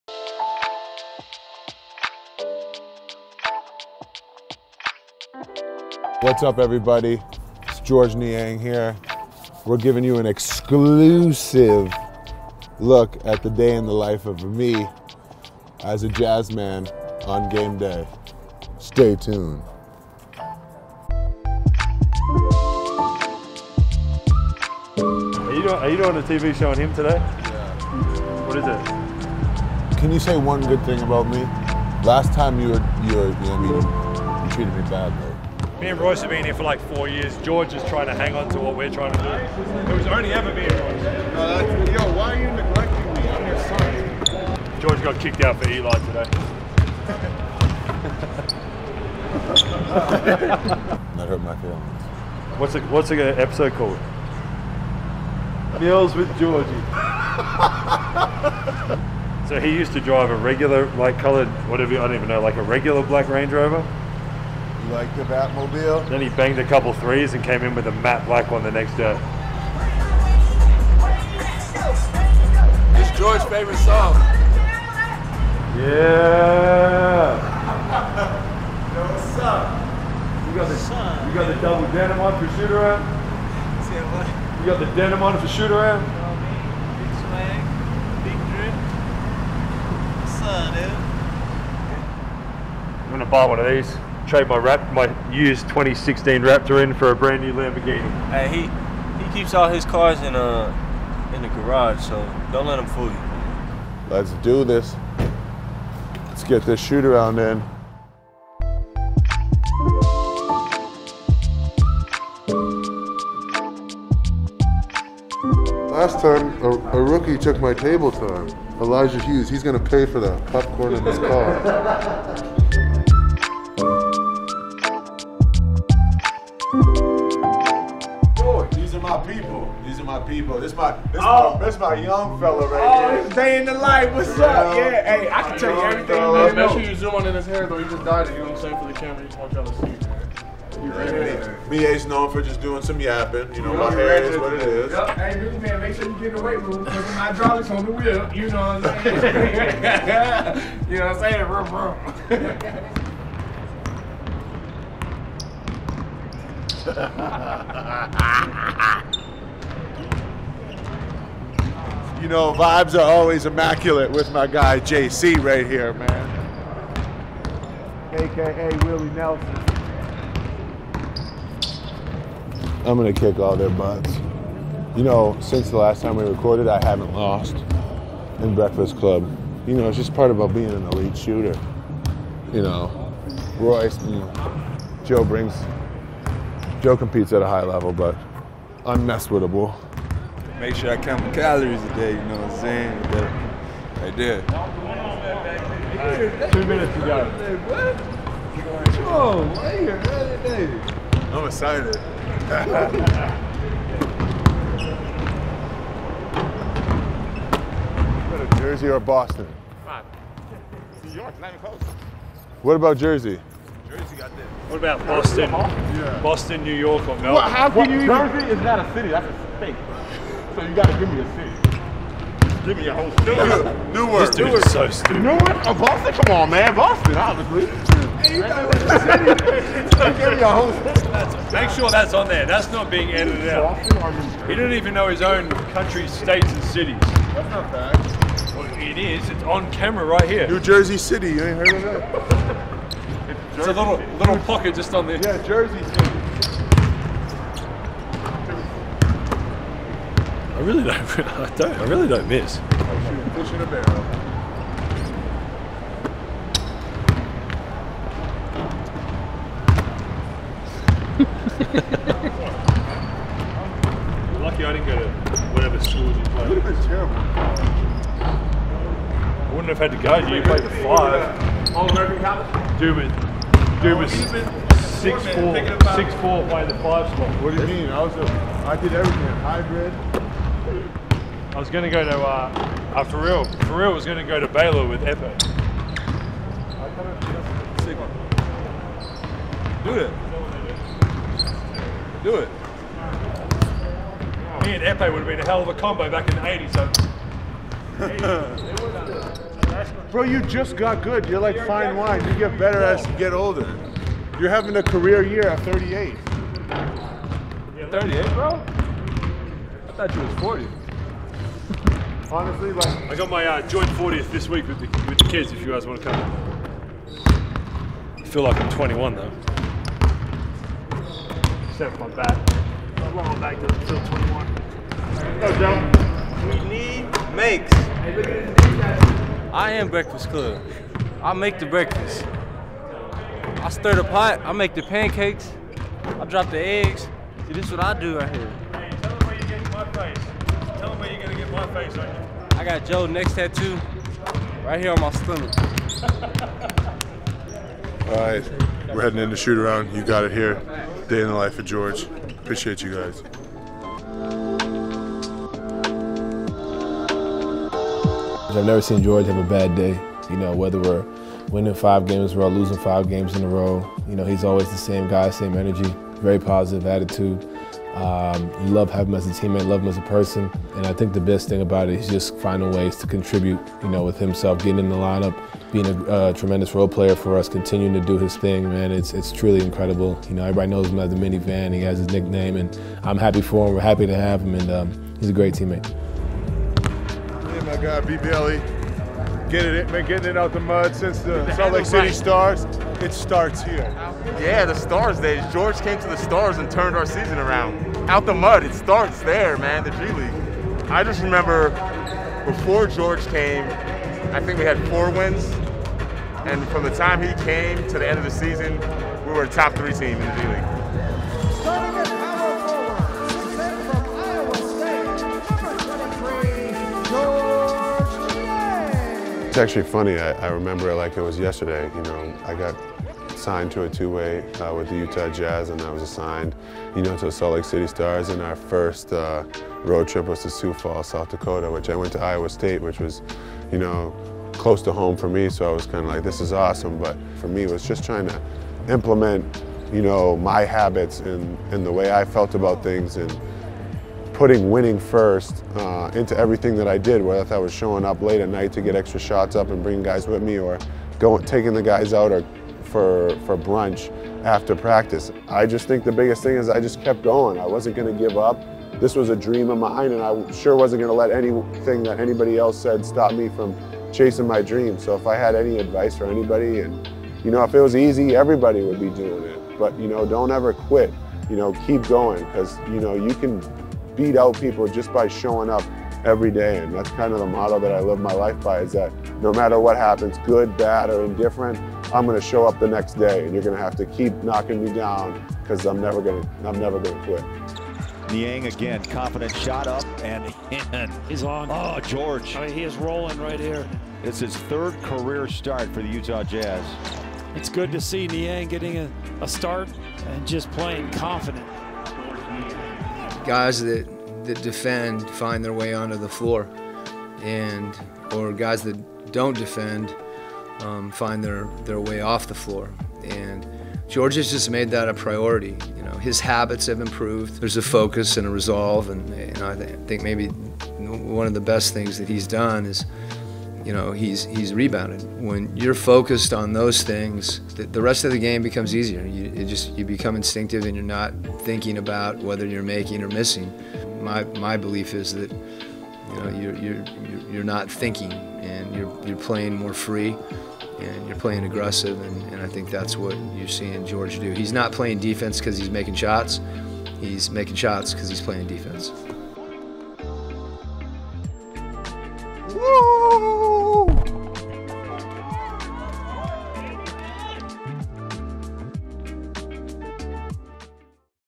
Georges Niang: Cyclone, SLC Star to Jazzman 🚐 | All-Access Presented by Vivint
Georges Niang went from Iowa State to being part of the G-League with the Salt Lake City Stars and has since becoming an important part of what the 1-seeded Utah Jazz roster.

Go behind-the-scenes with the Breakfast Club of Joe Ingles, Royce O'Neale and Jordan Clarkson and see what Quin Snyder says Niang brings to his team.